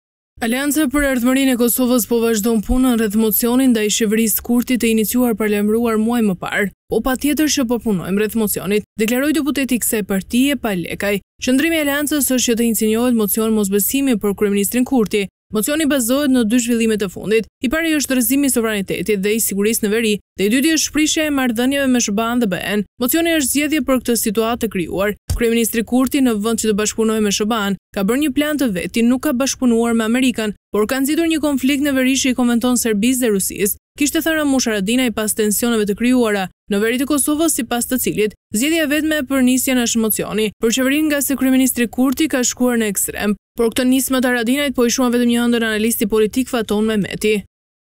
Çfarë po tenton Haradinaj me mocionin për rrëzimin e Kurtit?
Aleanca për Ardhmërinë e Kosovës po vazhdon punën rreth mocionit ndaj qeverisë Kurti të iniciuar paralajmëruar muaj më parë.
 “Po patjetër që po punojmë rreth mocionit”, deklaroi deputeti i kësaj partie, Pal Lekaj.
“Qëndrimi i Aleancës është që të iniciohet mocion mosbesimi për Kryeministrin Kurti. Mocioni bazohet në dy zhvillimet e fundit, i pari është rrezikimi i sovranitet dhe i sigurisë në veri dhe i dyti është prishja e marrëdhënieve me ShBA dhe BE. Do të fillojmë diskutimet me spektrin politik me shoqërinë në përgjithësi. Besoj që mocioni është zgjidhje për këtë situata të krijuar. Kryeministri Kurti në vend që të bashkëpunoj me ShBA, ka bërë një plan të vetin, nuk ka bashkëpunuar me Amerikën po ka nxit një konflikt në veri që i konvenon Serbisë dhe Rusisë”, kishte thënë Ramush Haradinaj pas tensioneve të krijuara në veri të Kosovës, sipas të cilit, zgjidhja e vetme është nisja e mocionit për Qeverinë ngase kryeministri Kurti ka shkuar në ekstrem.
Por ketë nismë të Haradinajt po e quan vetëm një ëndërr analisti politik, Faton Mehmeti.

𝐀𝐮𝐭𝐨𝐫 : NTV